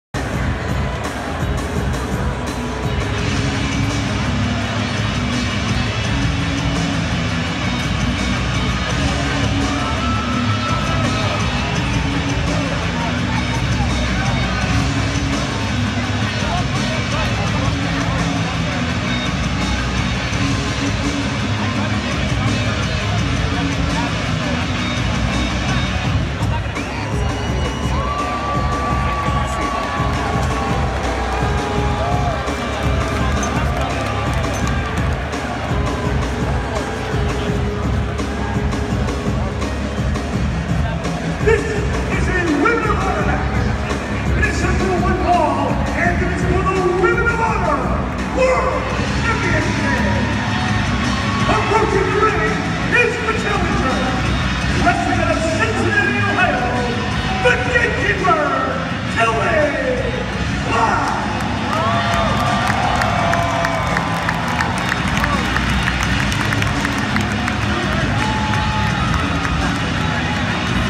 Kelly Klein Entrance at G1 Supercard.
This was Kelly Klein entrance at ROH/NJPW G1 Supercard.